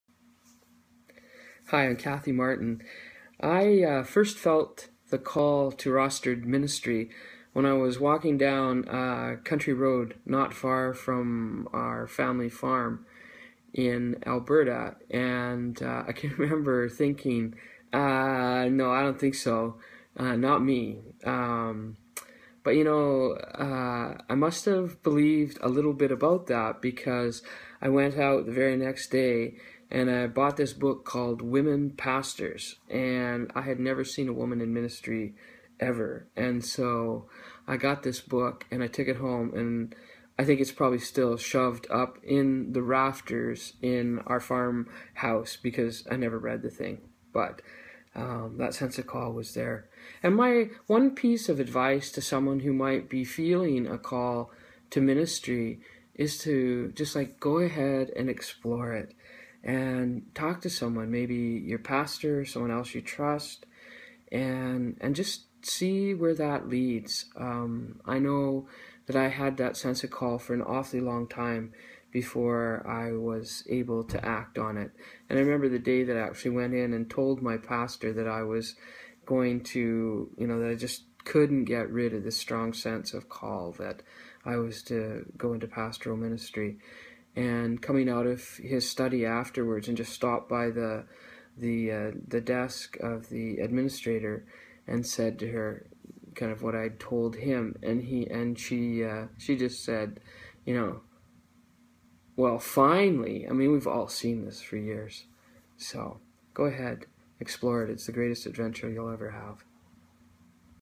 VocationalSunday - Rev. Kathy Martin
Rev. Kathy Martin shares when she first felt the call to Rostered Ministry.